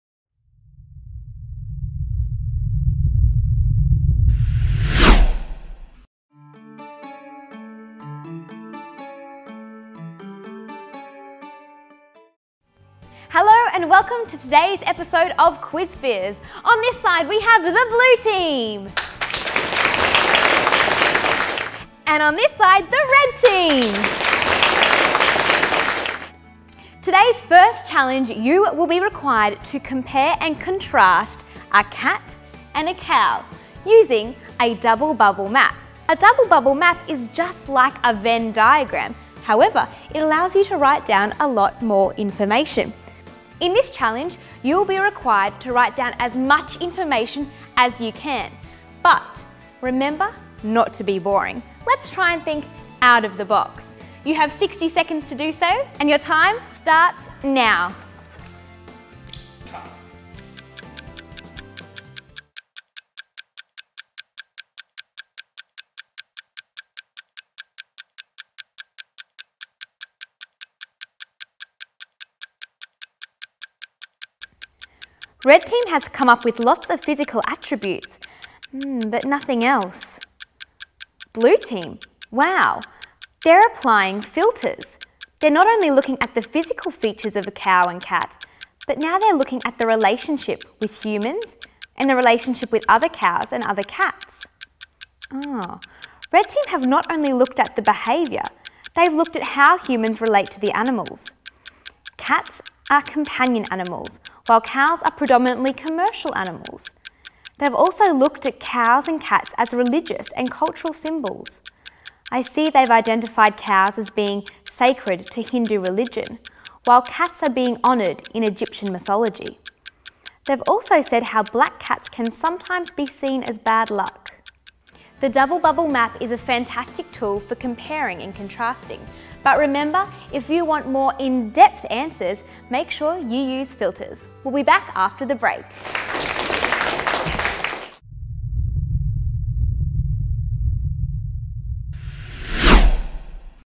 Double Bubble Maps
Why not join us at our national conference 18/19 July 2014?  See our website for more information.

The Double Bubble Map is an excellent tool to compare and contrast any two characters, policies, objects, stories or viewpoints. The graphic layout of the Double Bubble Map allows students more writing space than similar "compare and contrast" maps such as the Venn Diagram.

This is a great tool you can use to compare and contrast any two characters, objects, policies, stories or viewpoints.